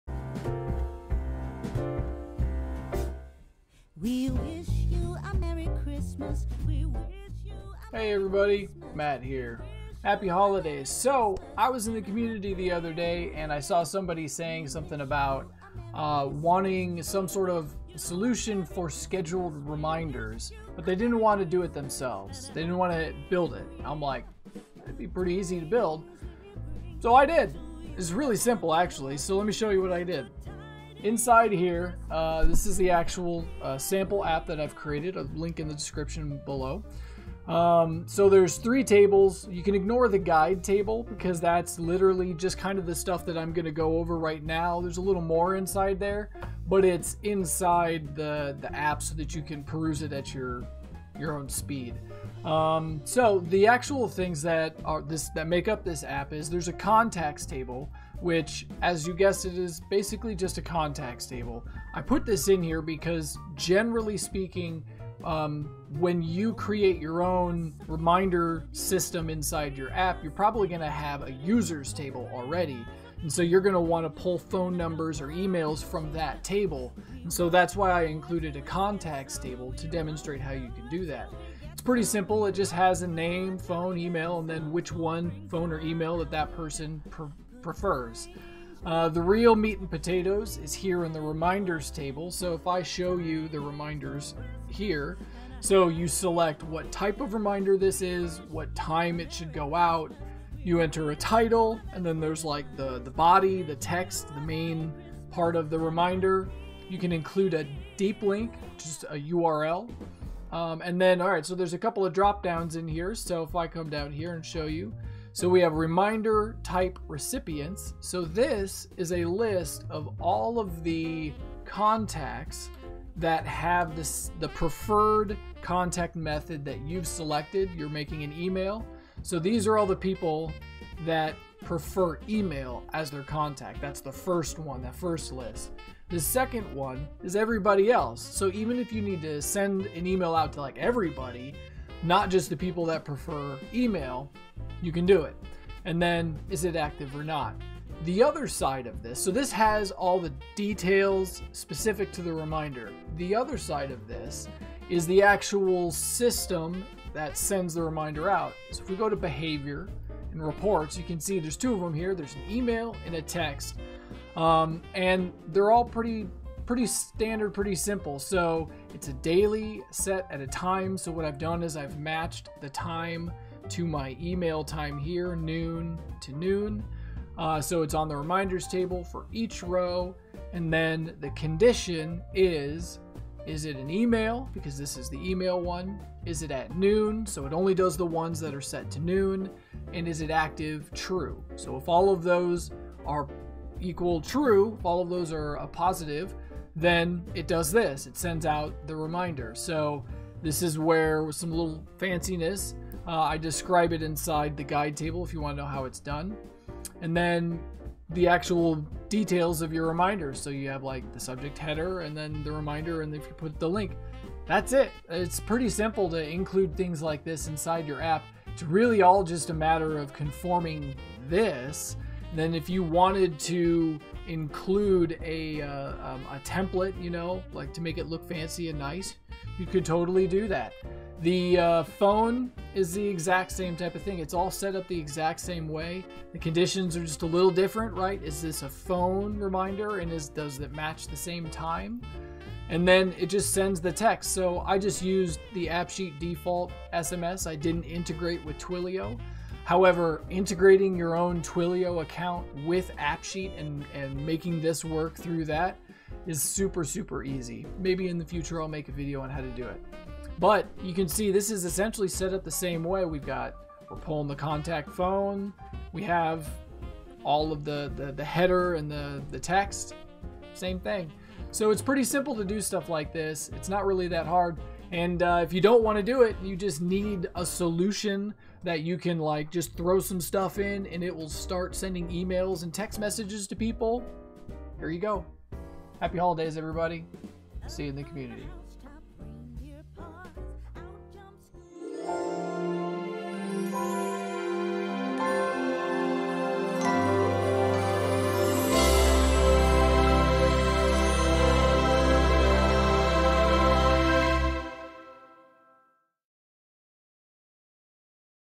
Scheduled Reminders || Sample App
I was in the AppSheet community and ran across a post someone had made requesting a solution for scheduled reminders.  They didn't want to build it themselves... so I did! (^_^)

or

 --For quick/easy/awesome videos
 --App promo videos, product videos, commercials, etc.